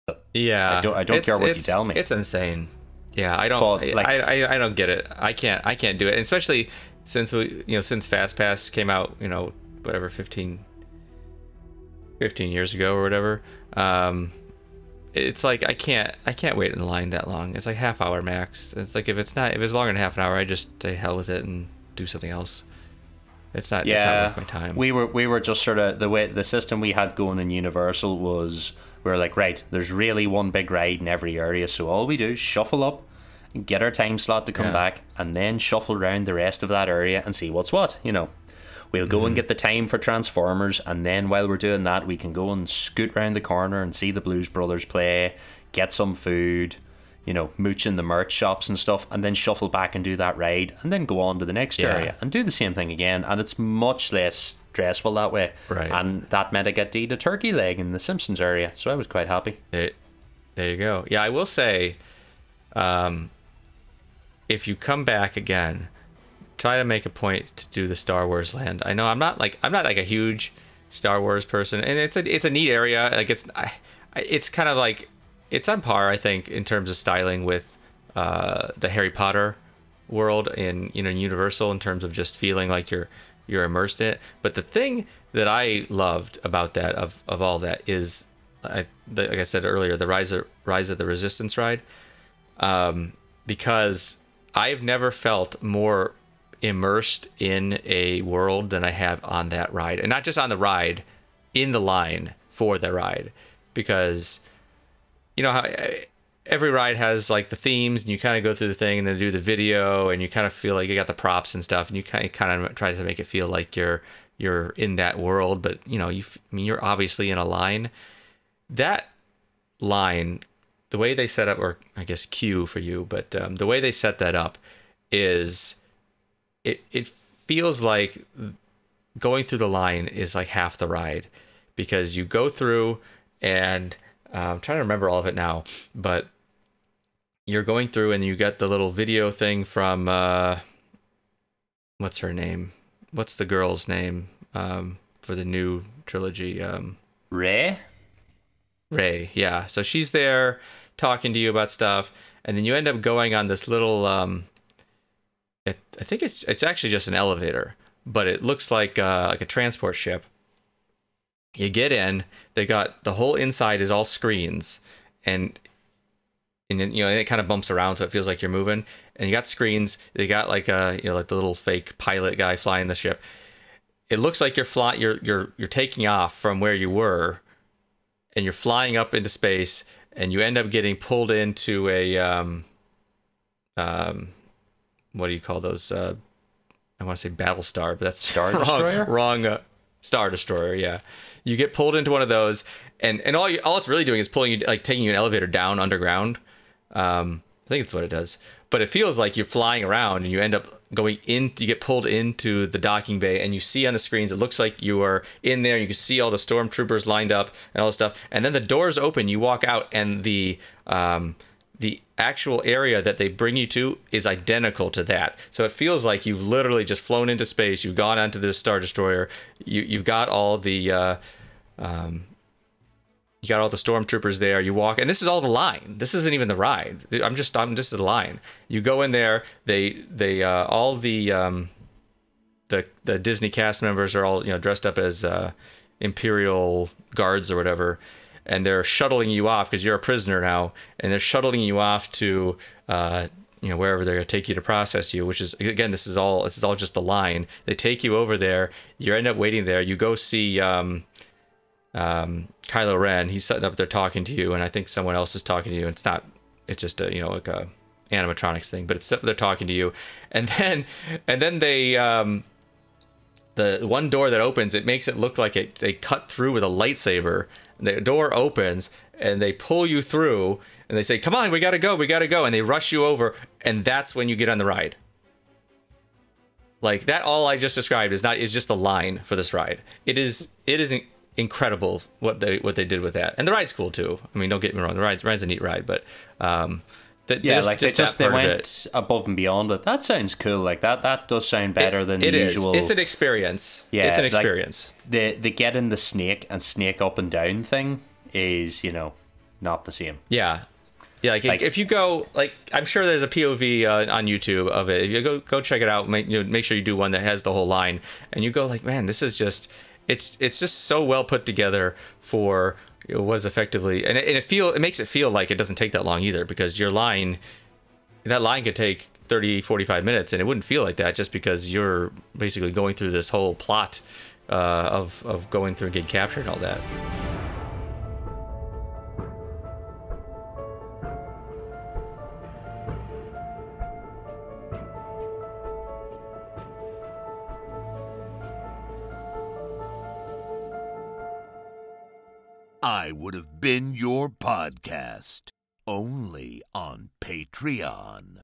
IWHBYP Preview | Episode 010 - Aaron World Showcase
Ian & Aaron discuss Aaron's recent trip to the states.